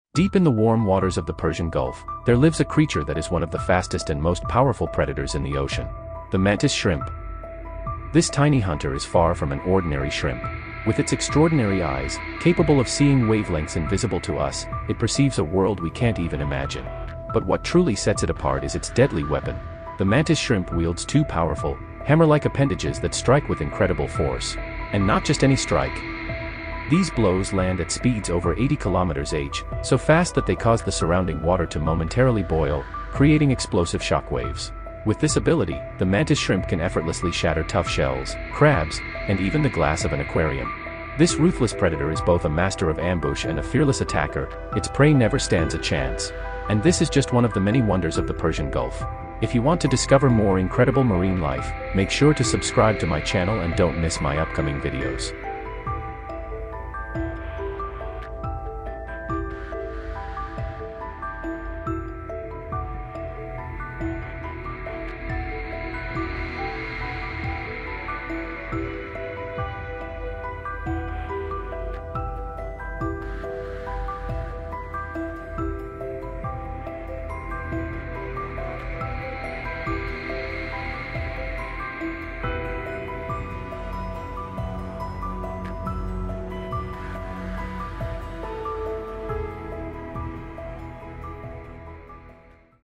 Mantis Shrimp of the Persian Gulf – Nature’s Ultimate Underwater Warrior 🦐
Description:
Beneath the waves of the Persian Gulf, a tiny but lethal predator reigns supreme—the mantis shrimp! Don’t be fooled by its size; this creature possesses one of the most powerful strikes in the animal kingdom, capable of smashing through shells, crabs, and even aquarium glass with the force of a bullet!

With lightning-fast reflexes, superhuman vision, and shockwave-inducing punches, the mantis shrimp is a true marvel of evolution. In this video, we dive deep into its incredible abilities, uncovering the science behind its deadly blows and the unique way it sees the world in colors beyond human perception.

🌊 Prepare to be amazed by one of the ocean’s most extraordinary hunters!